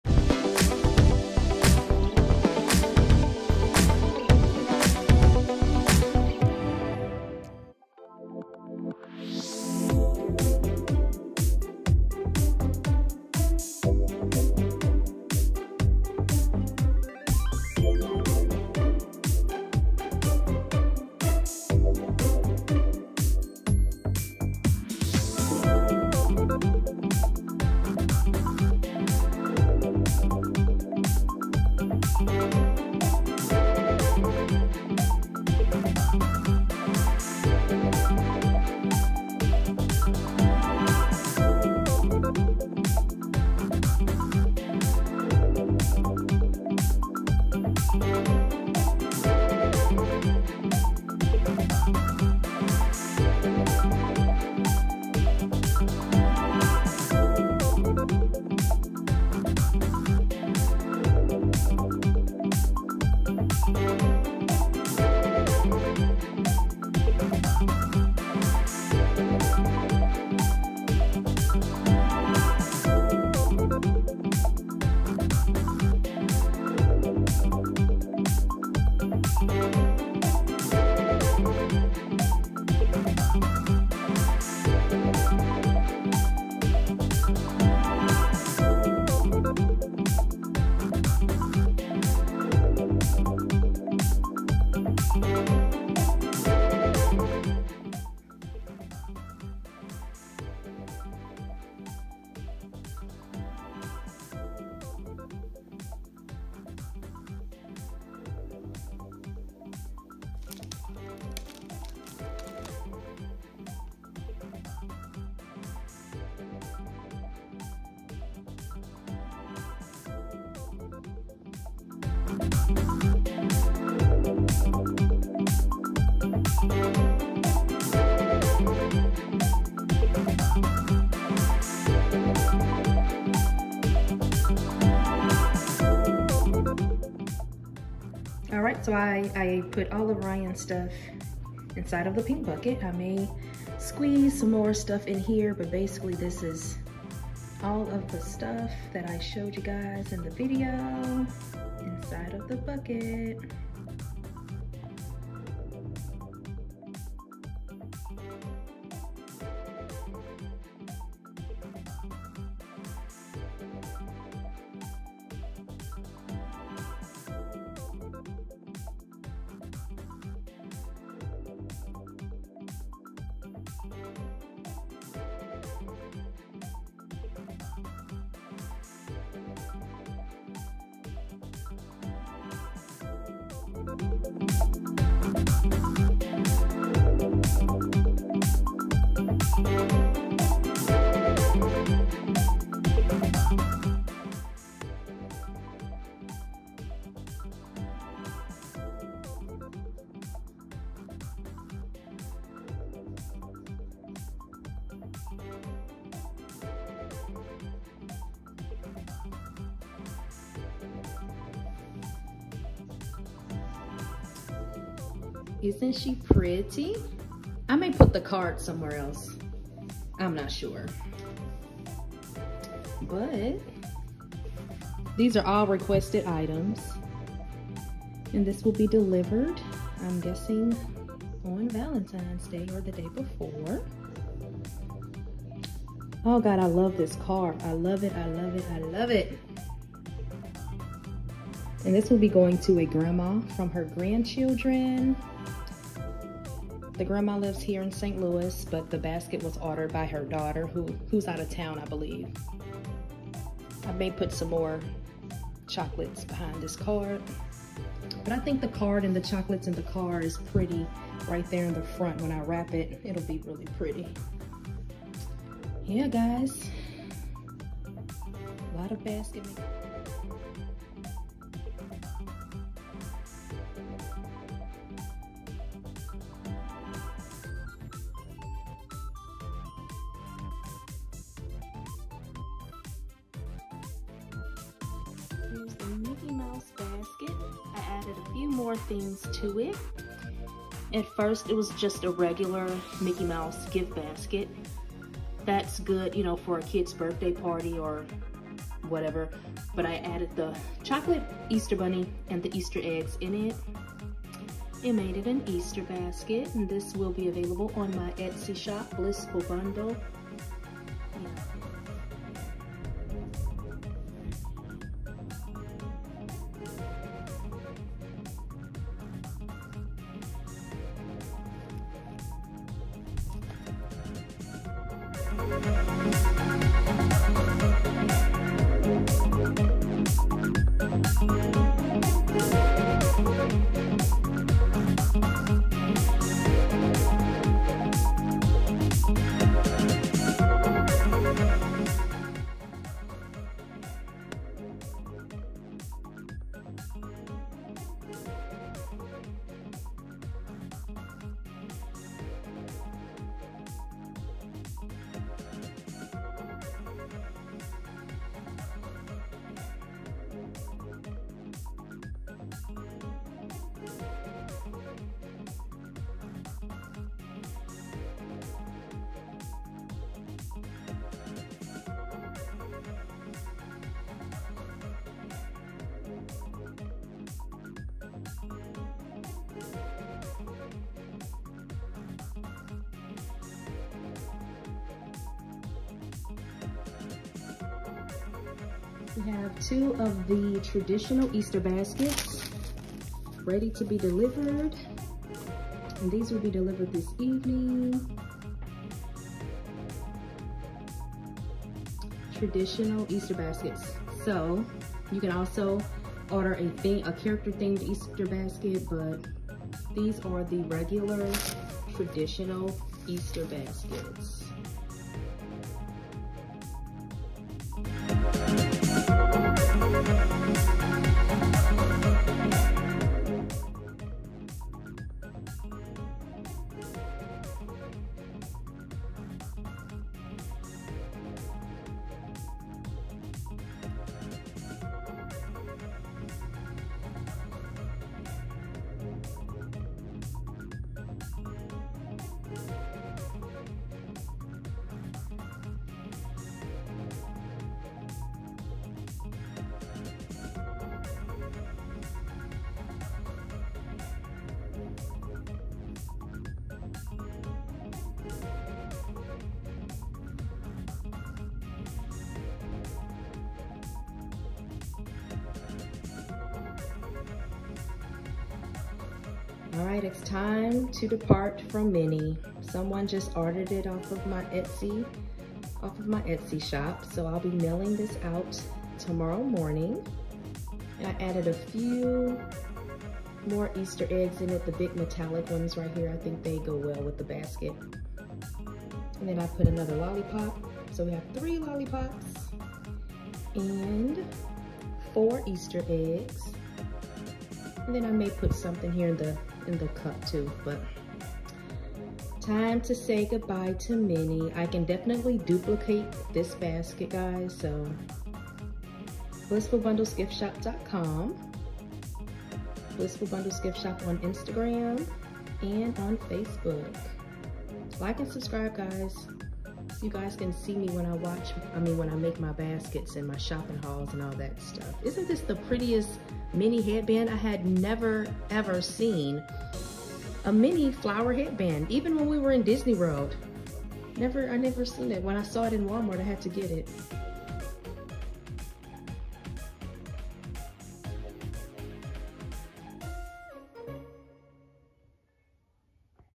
Easter baskets, Gift baskets, and more! A few of my basket creations!
I’m just a relatable mother juggling life, an acting career, gift shop business! Take the entrepreneurship, life style, and motherhood journey with me!

Blissful Bundles Etsy shop